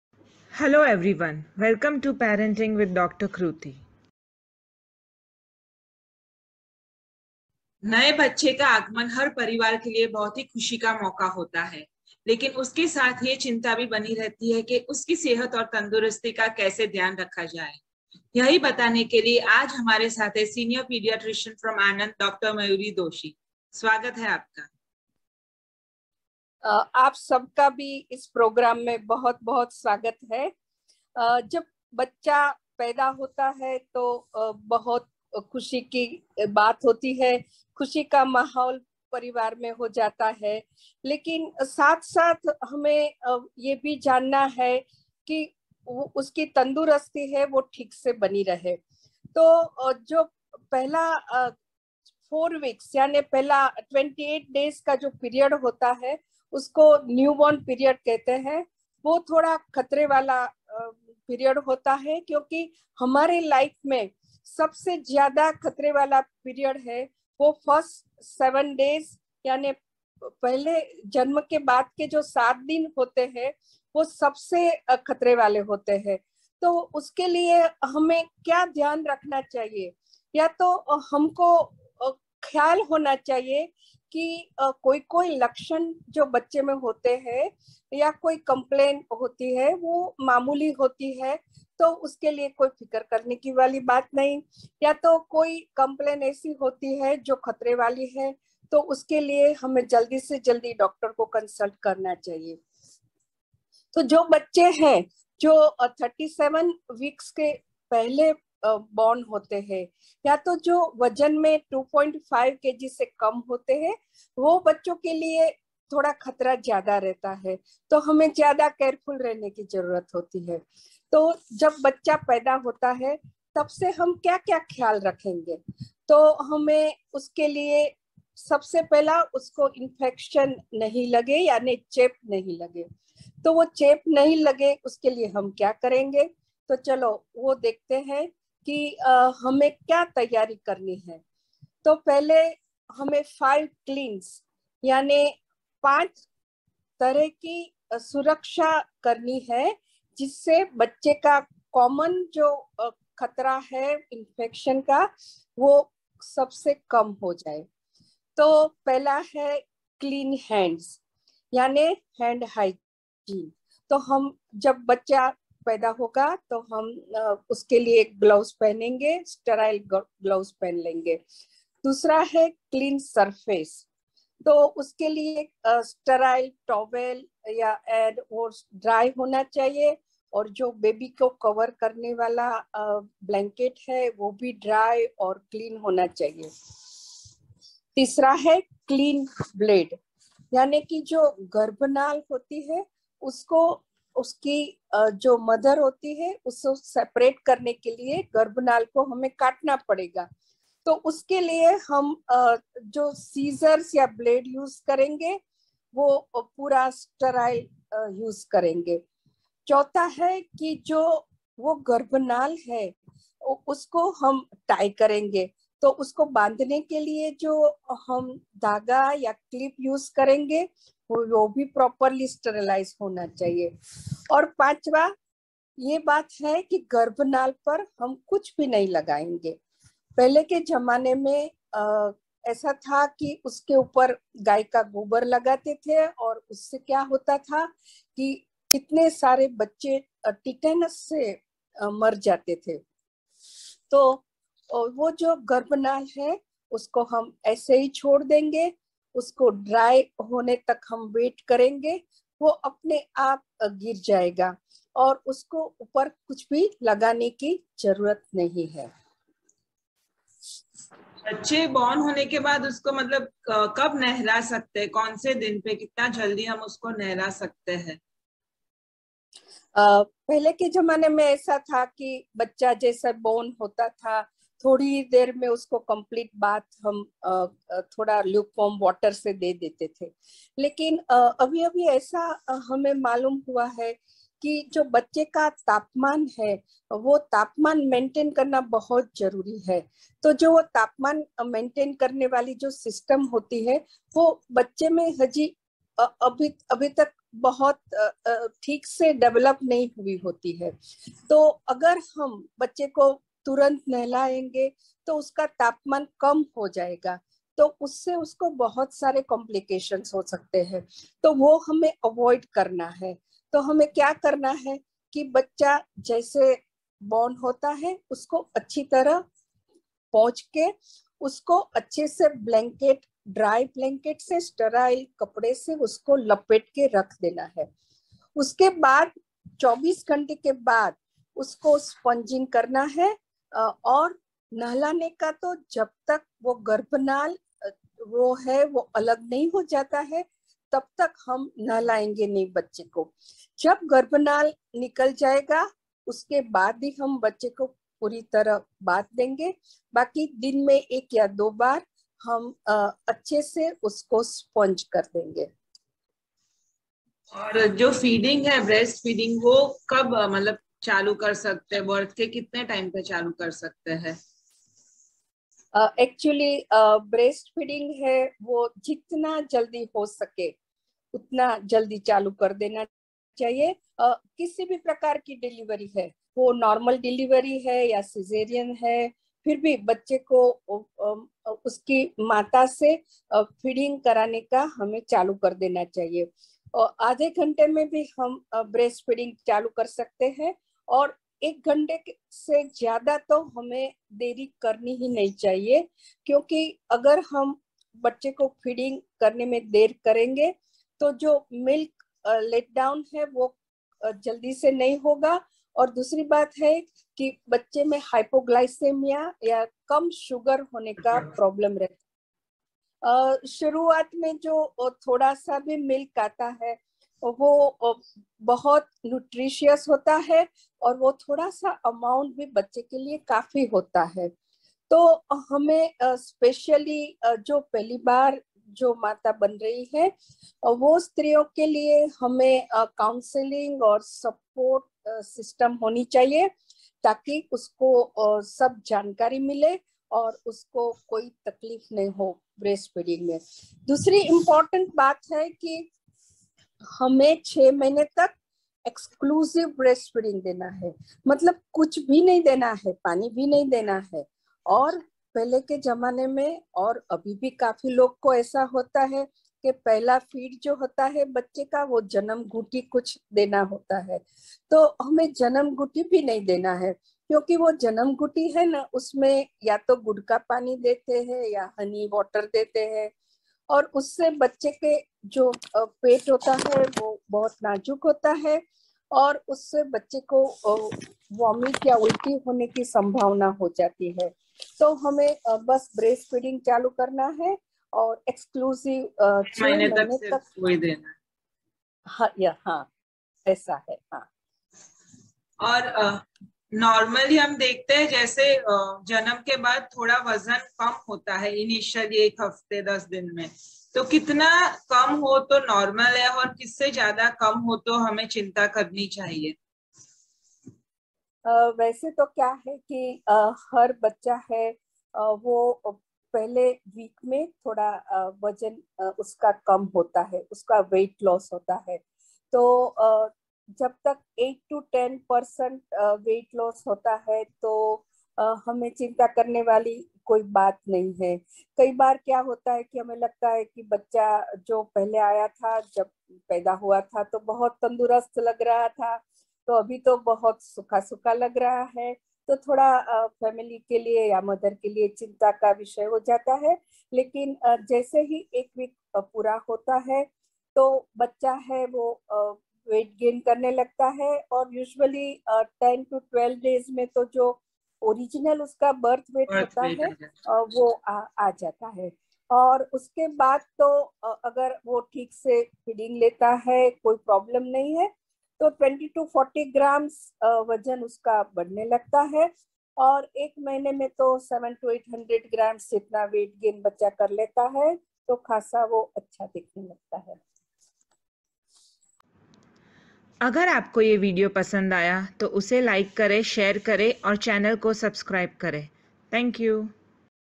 How to Take Care of Newborn With Dr Kruti & Dr Mayuri Part 01 | Parenting with Dr Kruti
Senior pediatricians Dr Mayuri Doshi talks with us about common queries related to newborn, what is normal and what are the danger signs.